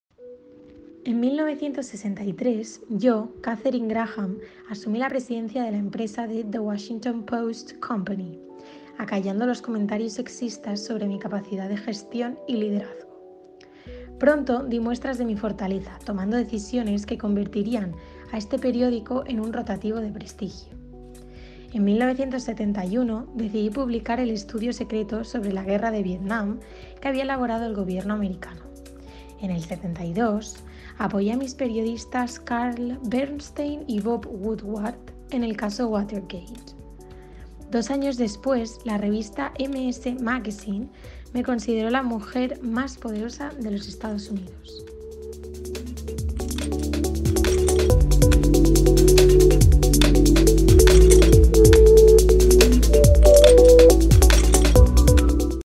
Katharine Graham, una mujer impulsora de la innovación en los medios de comunicación
“Las mujeres, impulsoras de la innovación” es un recorrido digital en el que 16 jóvenes de hoy dan voz a 16 mujeres que han tenido mucho que decir en su ámbito de conocimiento, como Katharine Graham. Una exposición virtual organizada por la Unidad de Igualdad y la Biblioteca-CRAI de la Universidad CEU Cardenal Herrera.